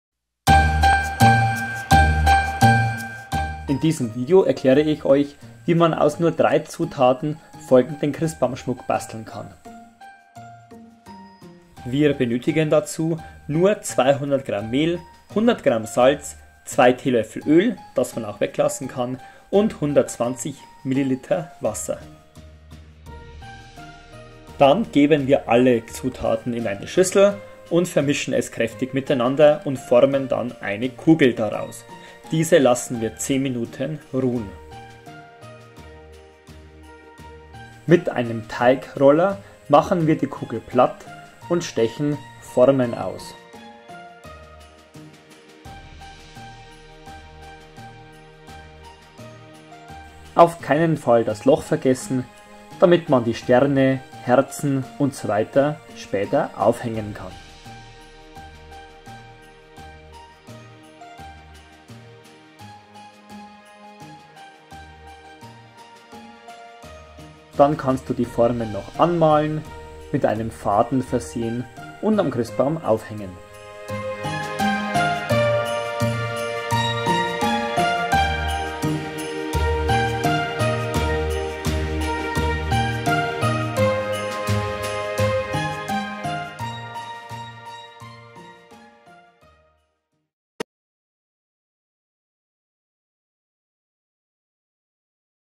Christbaumschmuck aus Salzteig | Weihnachtsbaumschmuck aus Salzteig basteln
Einfach, schnell und kreativ :-)
In diese Video zeige ich euch wie man aus sehr wenigen Zutaten Christbaumschmuck aus Salzteig basteln kann. Viel Spaß beim Anschauen und Backen :-)